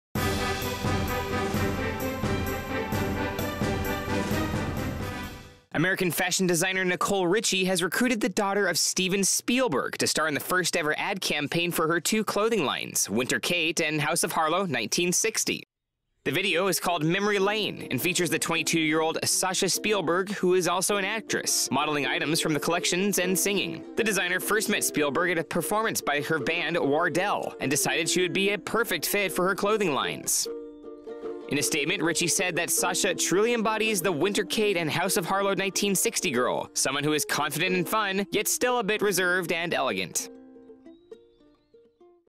Spielberg's daughter stars in advert: Sasha Spielberg working with designer Nicole Richie
American fashion designer Nicole Richie has recruited the daughter of Steven Spielberg to star in the first ever ad campaign for her two clothing lines, Winter Kate and House of Harlow 1960.